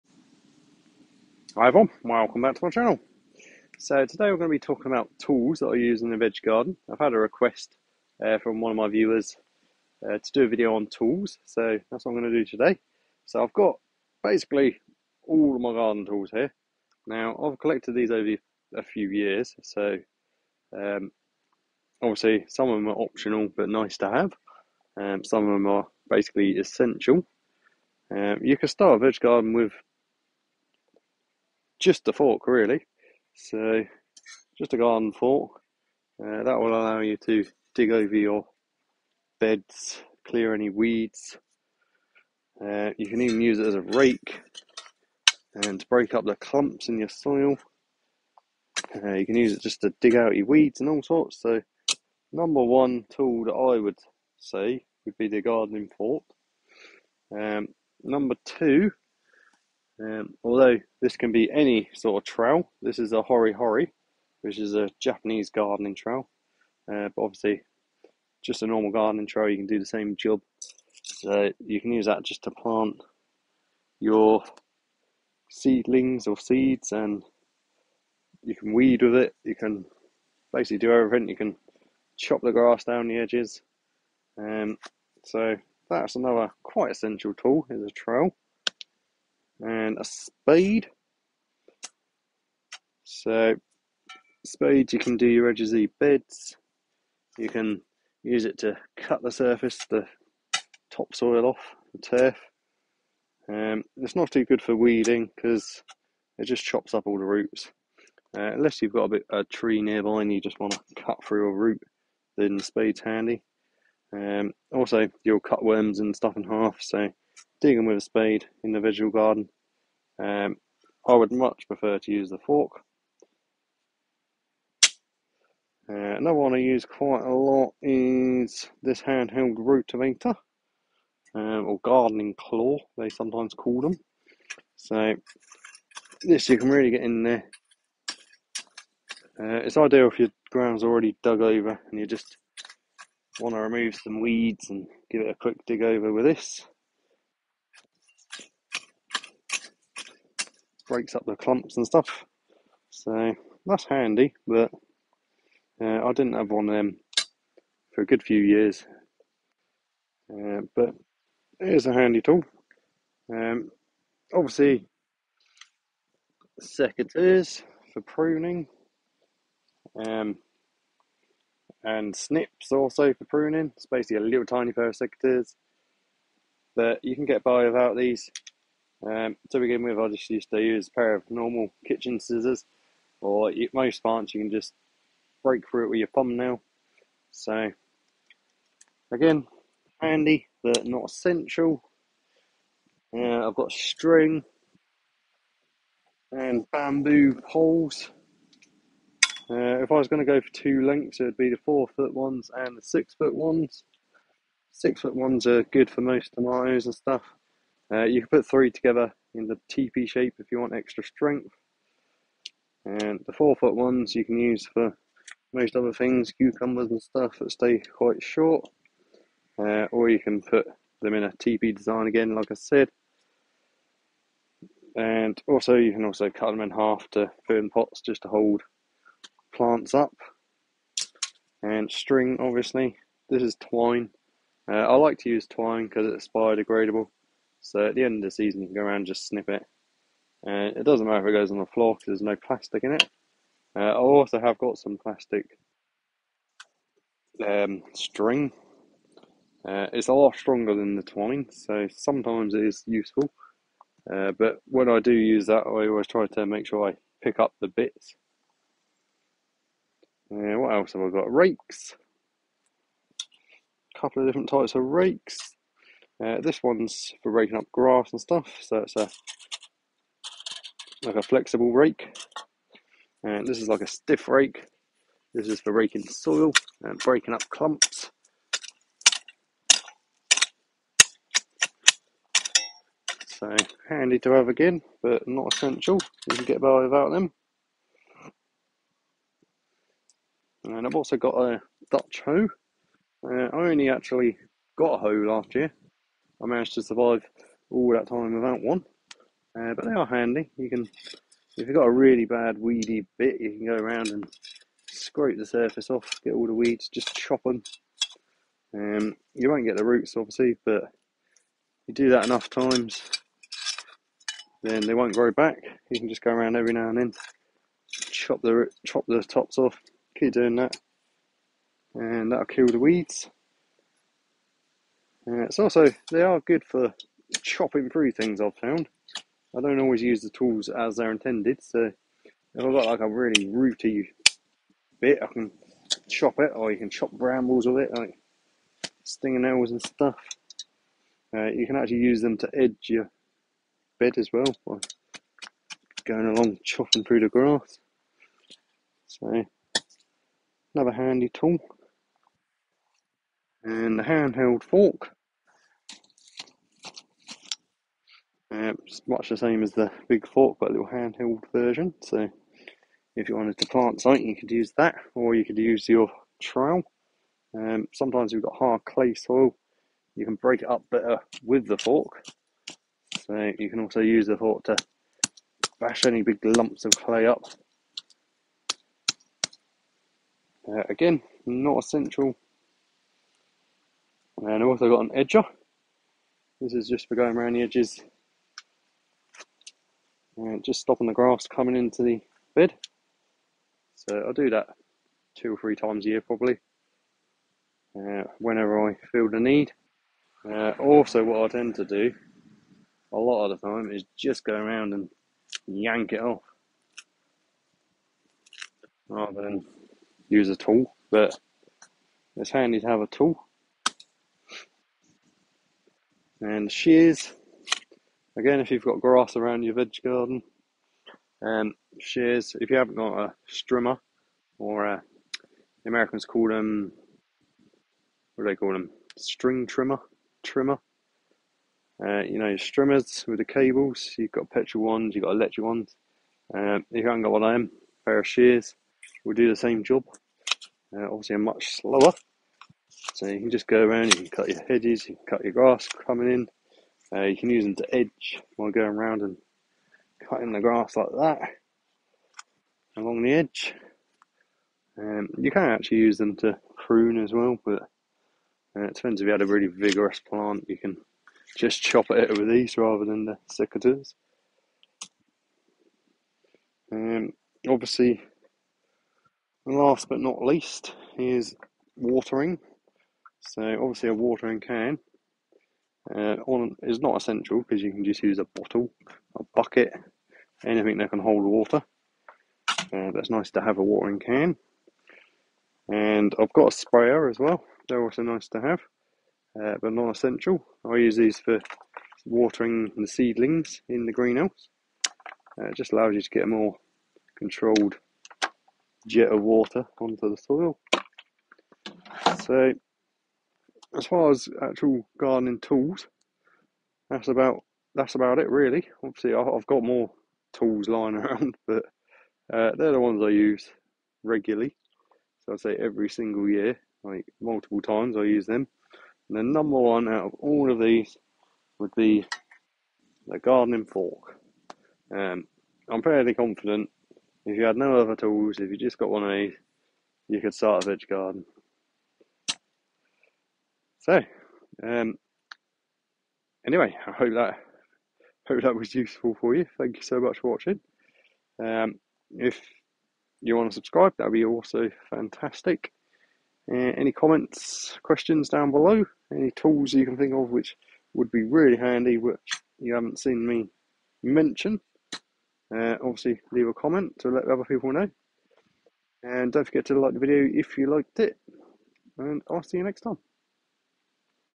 Essential Gardening Tools for beginners | Veg Gardening
hi everyone, today I’m talking about Gardening tools for beginners to start their veg garden.
I’ll talk about the best garden tools to start your veg garden and the nice to have but optional gardening tools that you can get in the future.
these are my opinions and I’d love to hear yours.